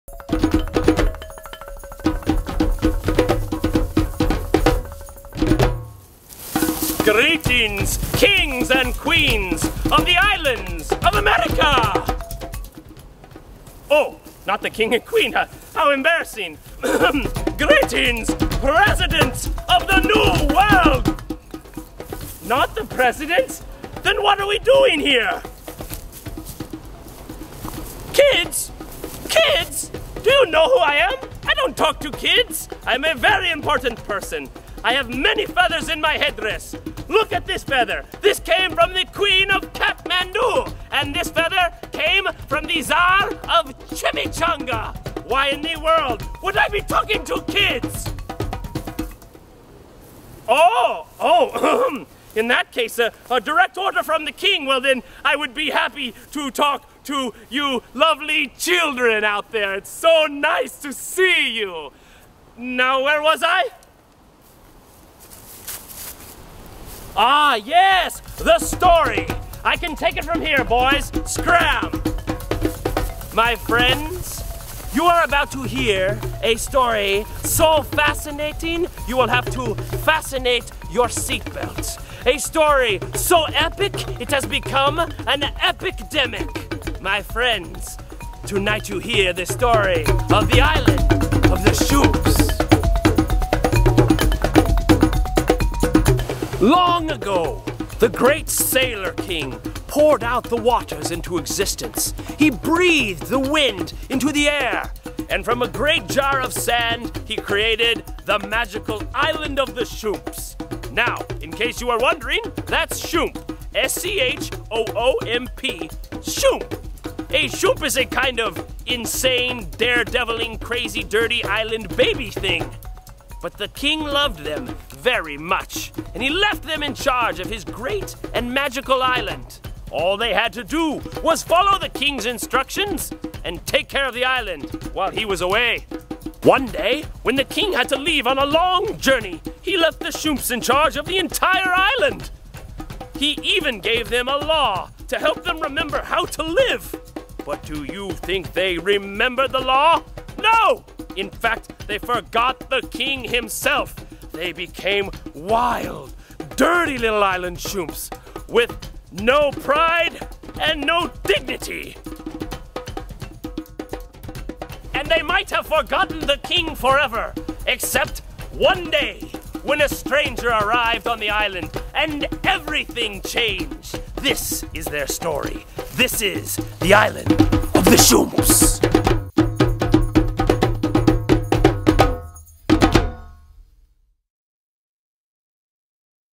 The Island of the Schoomps (opening Narration)
The story of how the shoomps became what they are today.